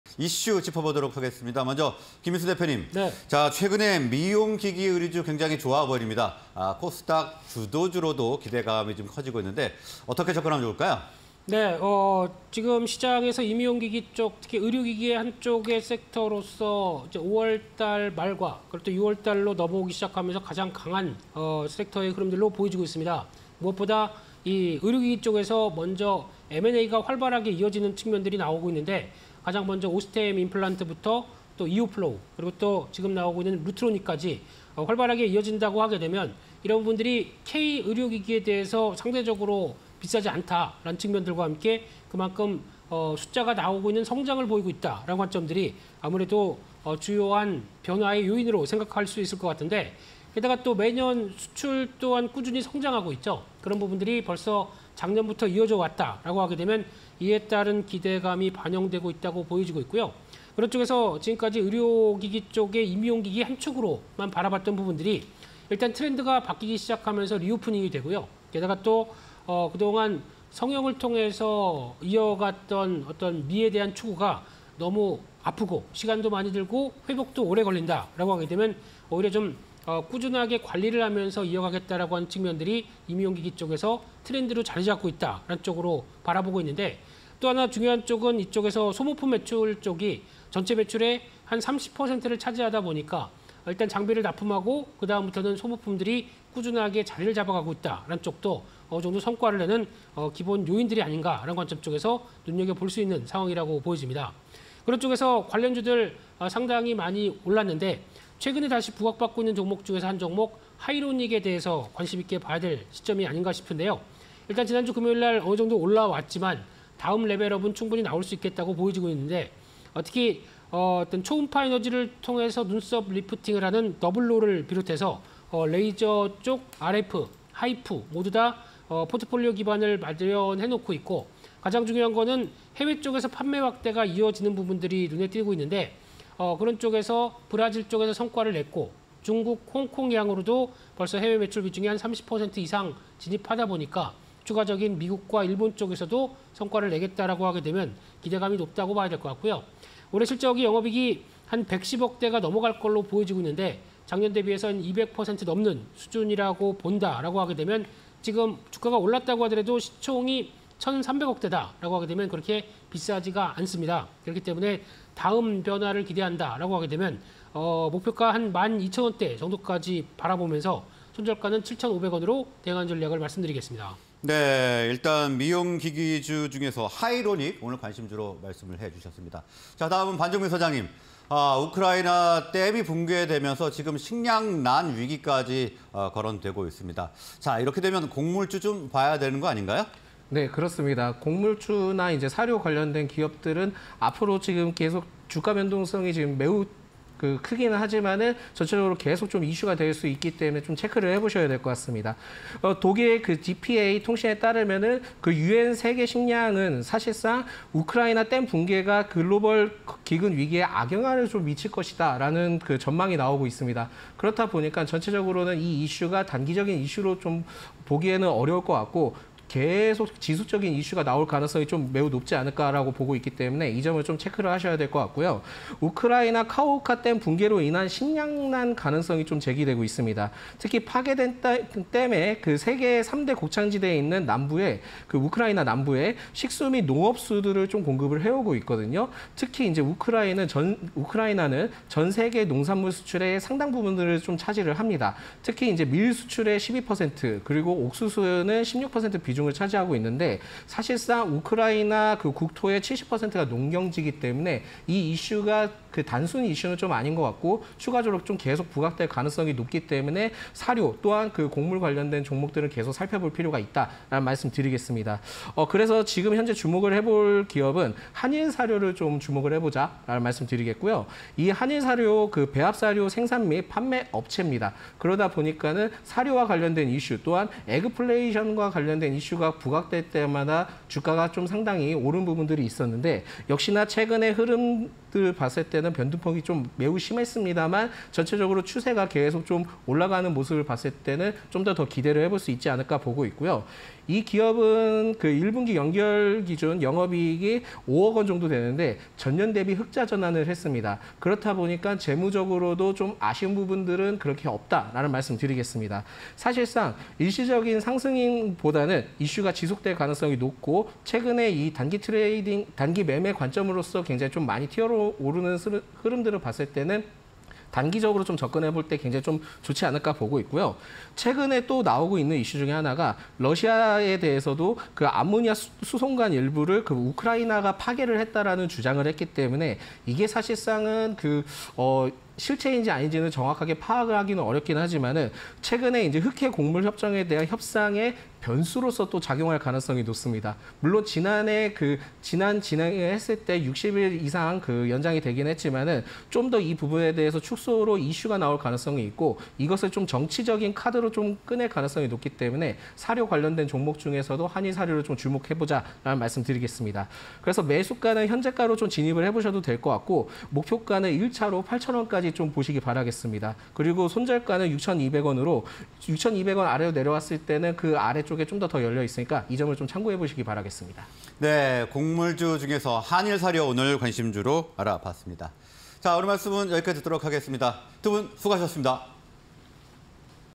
하이로닉(149980)… 브라질향 75억원 V-RO 장비 납품 / 한일사료(005860)… 1분기 영업익 5.06억원… 전년비 흑자전환_개장 관심주 (20230612)
Q. 미용기기株, 코수닥 주도주 꿰차나… 전략은?
Q. '우크라 댐 붕괴' 곡물주, 상승세 이어지나?

하이로닉(149980)
- DOUBLO 및 레이저·RF·HIFU 기반 포트
- 브라질향 75억원 V-RO 장비 납품
- 중국·홍콩향 해외 매출 비중 30% 이상
- 매출액 387억·영업익 114억 전망 VS 시총 1,300억

한일사료(005860)
- 배합사료 생산 및 판매 업체
- 1분기 영업익 5.06억원… 전년비 흑자전환
- 곡물가 상승 우려…테마 지속 유효